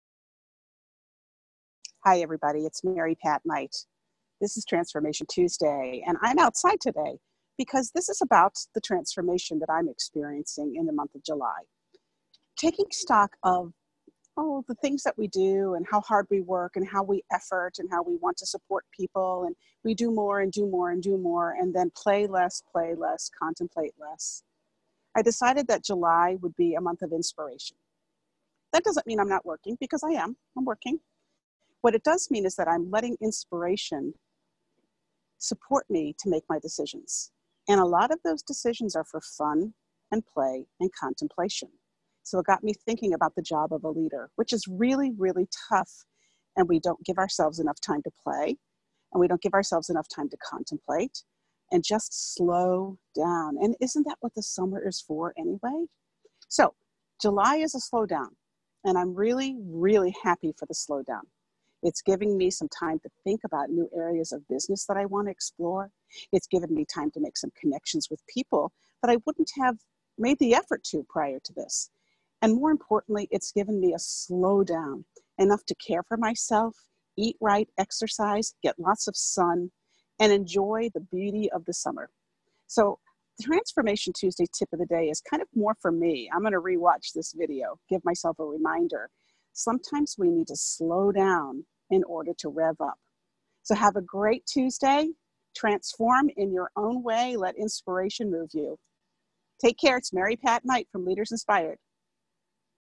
Slow Down to Rev Up | Transformation Tuesday
It's casual day at Leaders Inspired. Enjoy this TransformationTuesday video about the power of slowing down to rev up.